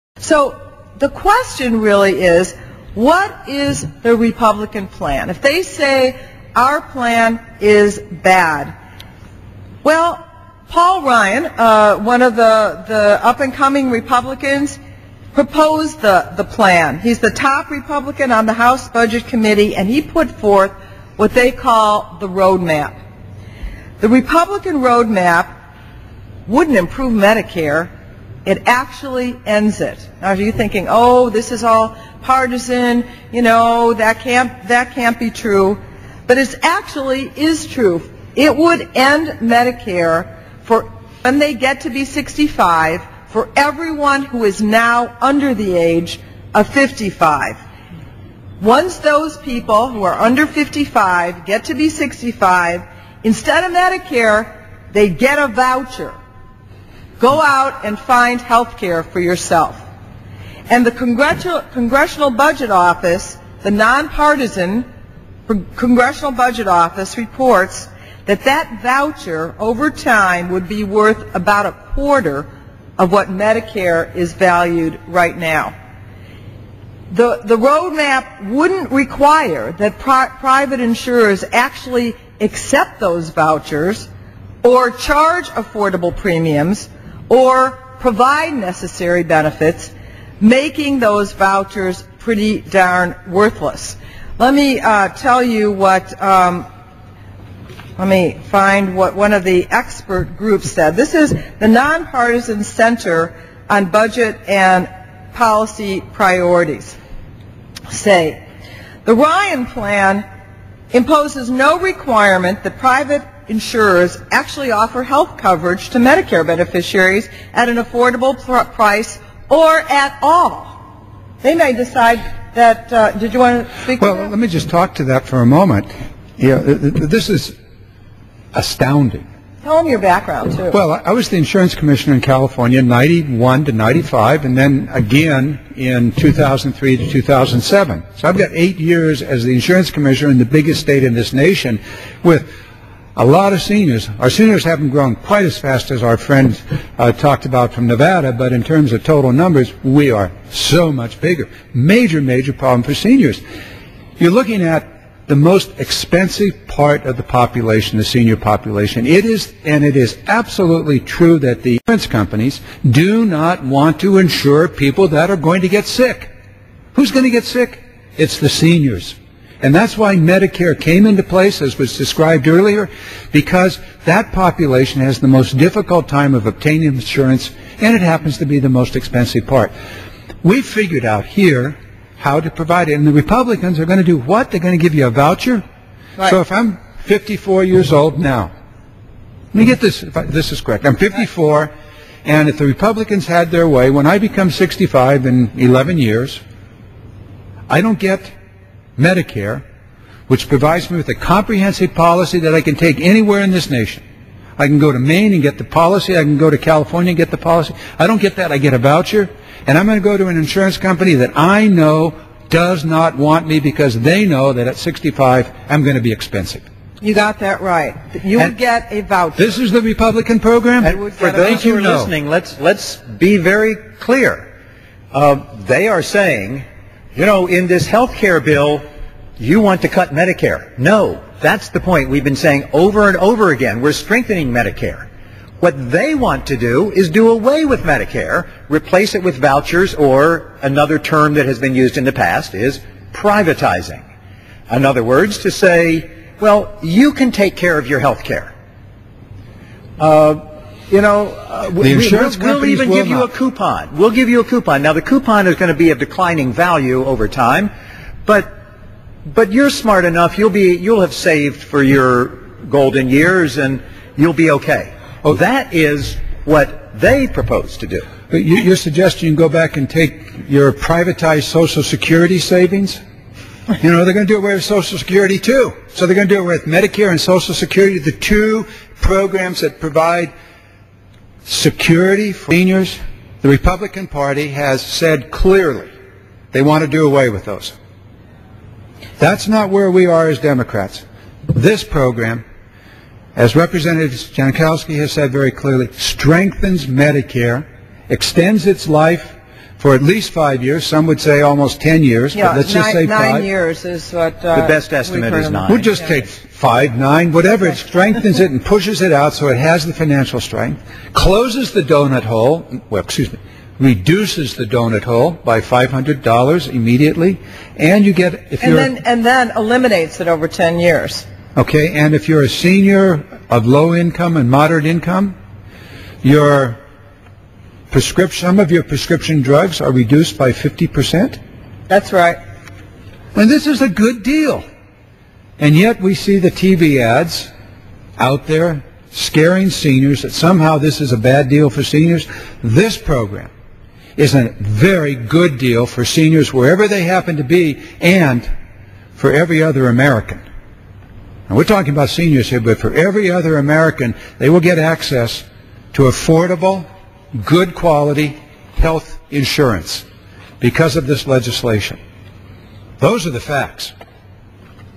Garamendi on health reform: This is a very good deal
On Wednesday, March 17th, Congressman John Garamendi (D-Walnut Creek) joined Representatives Jan Schakowsky (D-Illinois), Rush Holt (D-New Jersey), Dina Titus (D-Nevada), and Keith Ellison (D-Minnesota) to discuss the ways health care reform will help seniors.